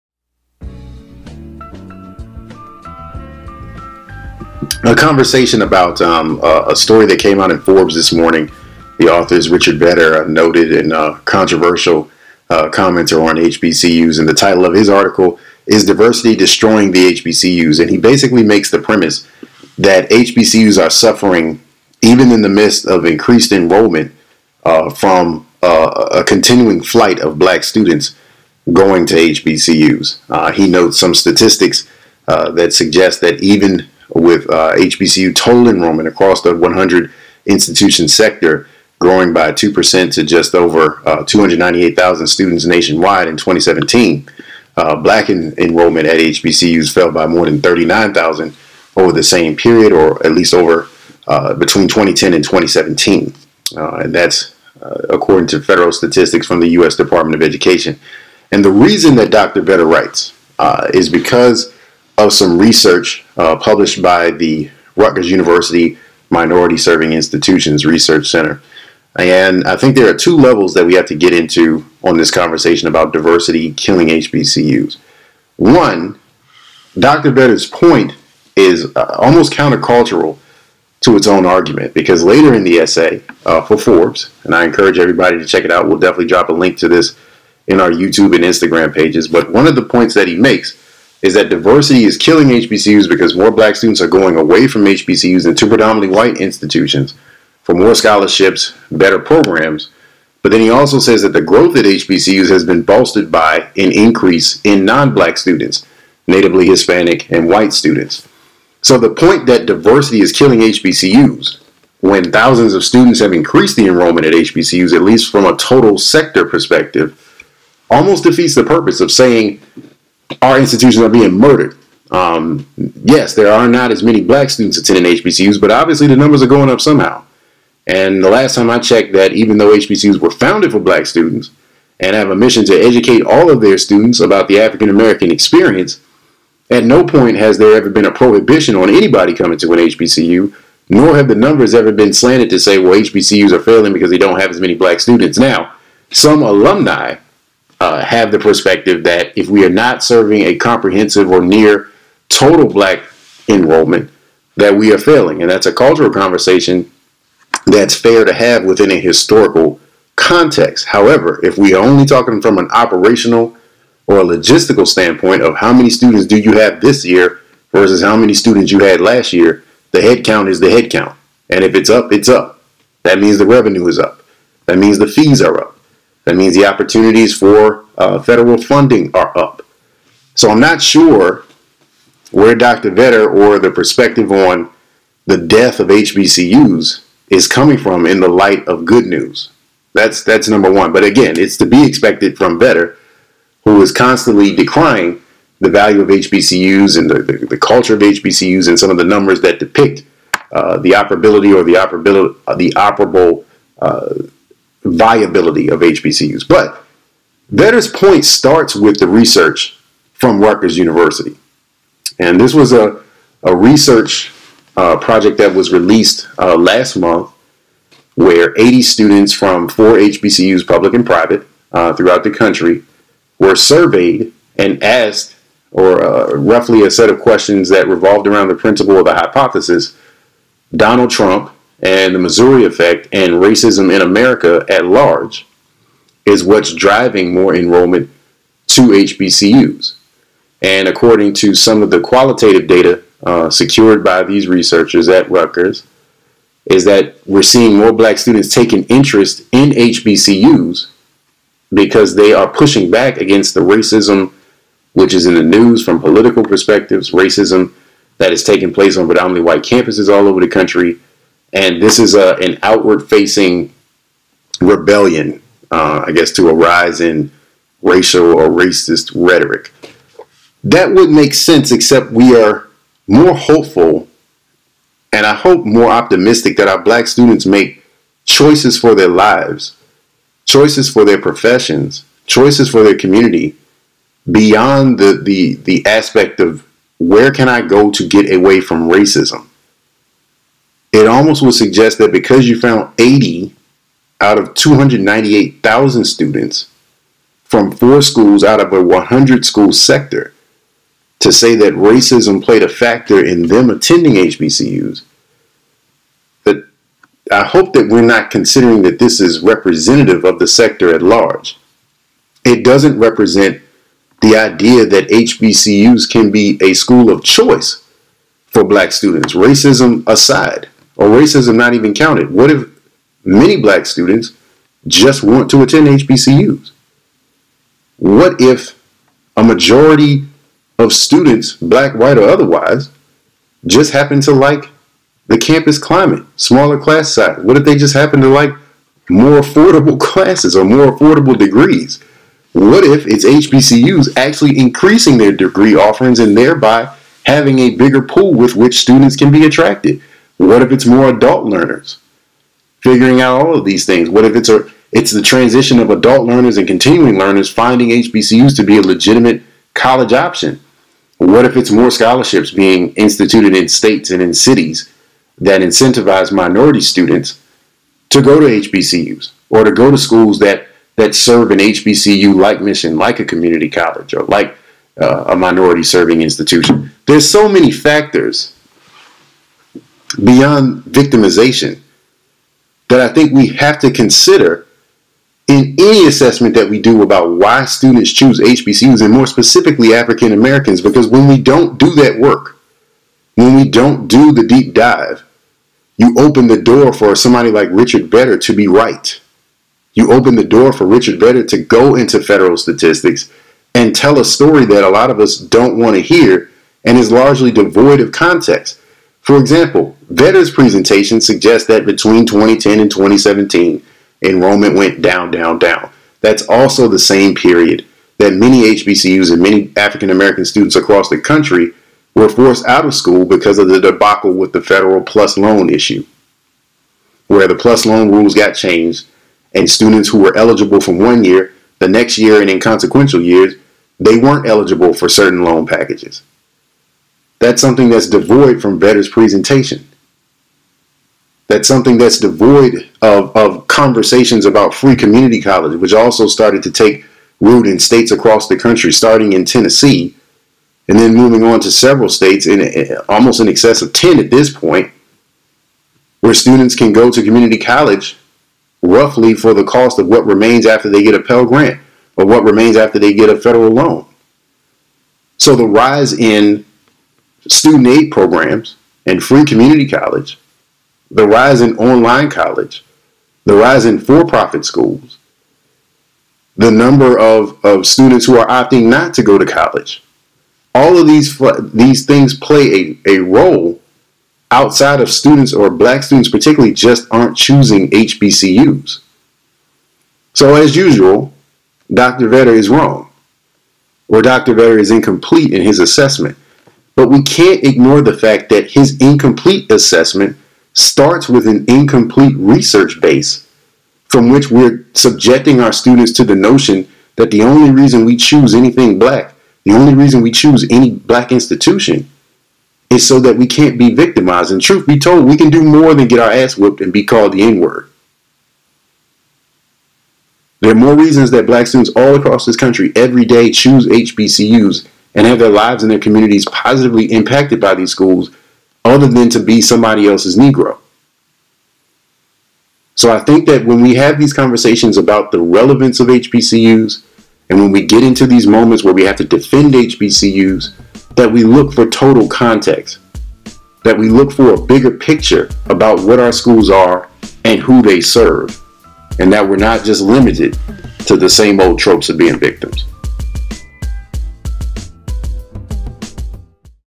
When Bad Research Fuels HBCU Stereotypes
Jarrett Carter Sr. shares thoughts on today's Forbes Magazine piece on how "diversity is killing HBCUs."

Forbes "Is diversity destroying the HBCUs?"